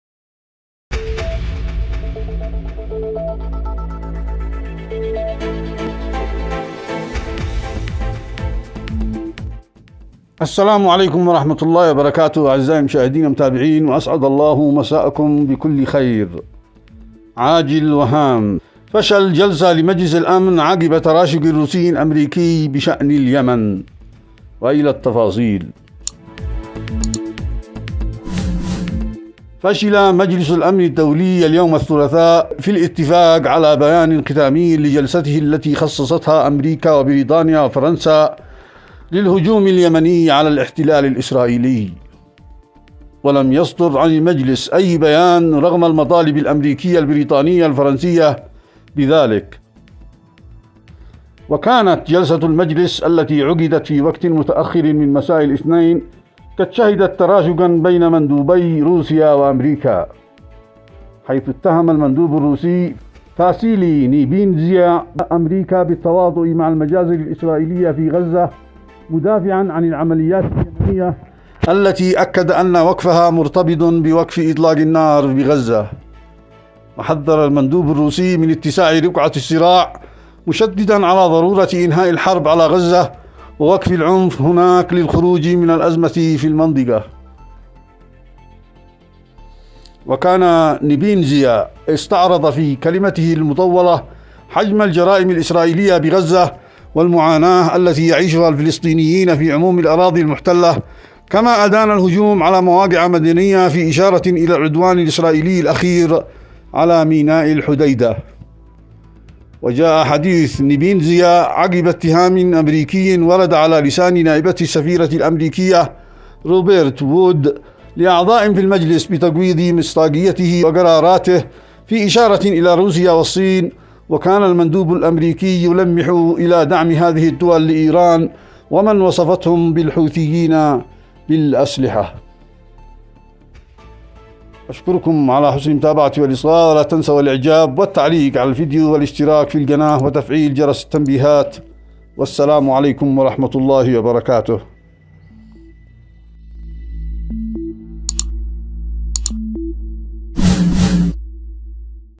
عاجل وهام | تراشق أمريكي روسي في مجلس الأمن بسبب اليمن
مسلسل ماء الذهب
اشتباكات_ البحر_الأحمر
مواجهات_ البحر_الأحمر